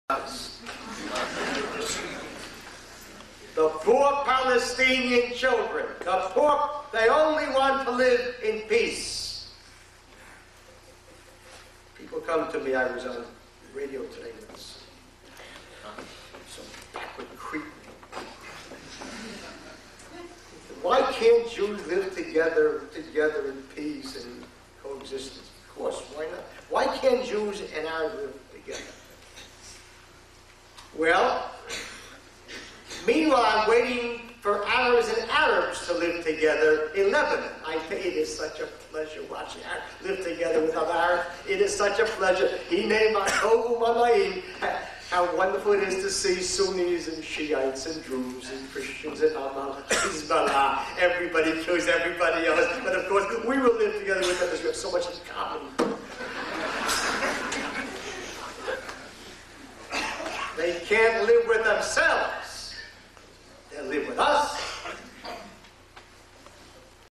15.10 Рабби Меир Кахане הי"ד Знаете, это так здорово - смотреть, как арабы "мирно" живут
- Знаете, это так здорово - смотреть, как арабы "мирно" живут с другими арабами. Как здорово видеть вместе суннитов, шиитов, друзов, христиан, "Амаля" и "Хезболлу". Каждый убивает каждого. Но естественно, мы можем с ними жить в мире, потому, что у нас так много общего!